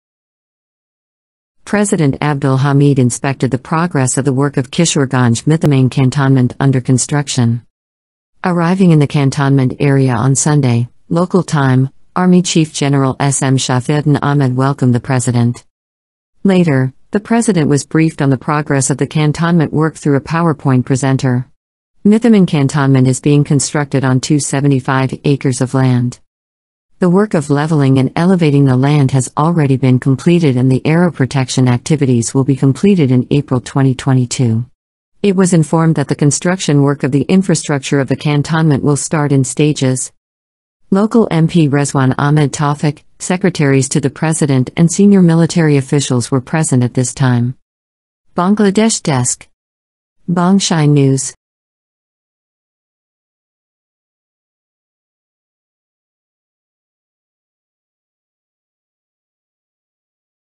President Visited The Cantonment Under Construction In Kishoreganj
President Abdul Hamid inspected the progress of the work of Kishoreganj Mithamain cantonment under construction.
Arriving in the cantonment area on Sunday, local time, Army Chief General SM Shafiuddin Ahmed welcomed the President.

Later, the President was briefed on the progress of the cantonment work through a PowerPoint presenter. Mithamin cantonment is being constructed on 275 acres of land.

The work of leveling and elevating the land has already been completed and the arrow protection activities will be completed in April 2022. It was informed that the construction work of the infrastructure of the cantonment will start in stages.

Local MP Rezwan Ahmed Tawfiq, secretaries to the president and senior military officials were present at This Time.

Bangladesh Desk
Bongshai News.